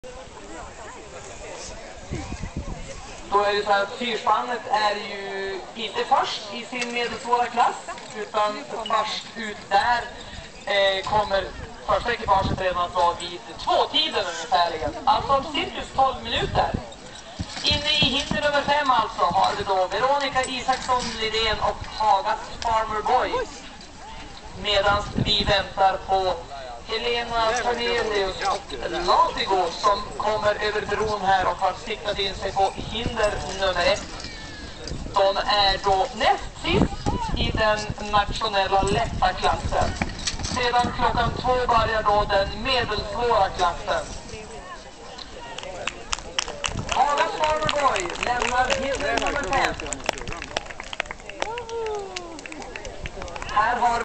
hagas farmer boy-dansbanan
Duktiga Farmarpojken kör järnet i dansbanehindret. Villvattnet 2010